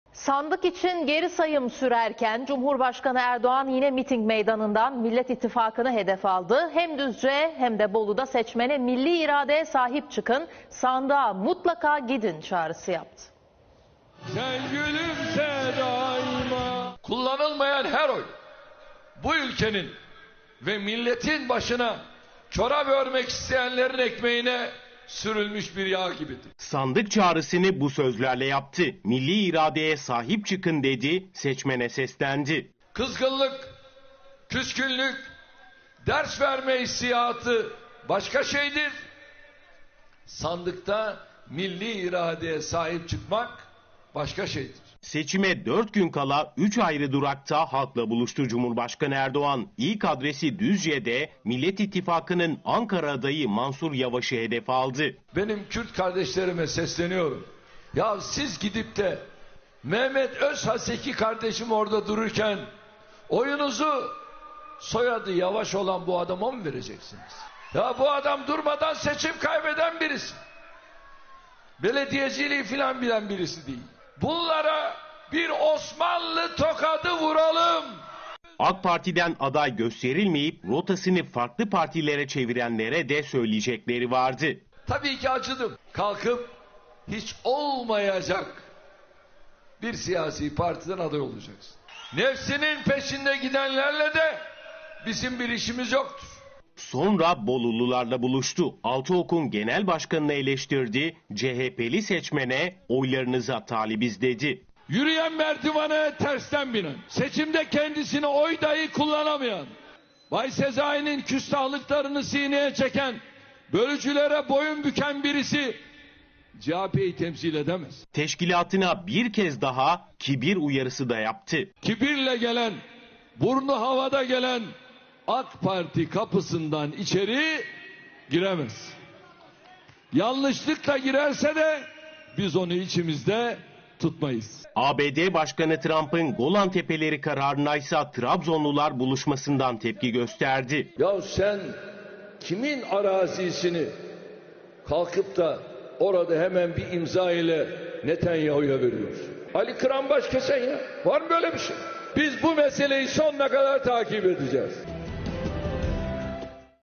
Cumhurbaşkanı'ndan sandık çağrısı
SANDIK İÇİN GERİ SAYIM SÜRERKEN, CUMHURBAŞKANI ERDOĞAN YİNE MİTİNG MEYDANINDAN MİLLET İTTİFAKINI HEDEF ALDI. HEM DÜZCE HEM DE BOLU'DA SEÇMENE "MİLLİ İRADEYE SAHİP ÇIKIN, SANDIĞA MUTLAKA GİDİN" ÇAĞRISI YAPTI.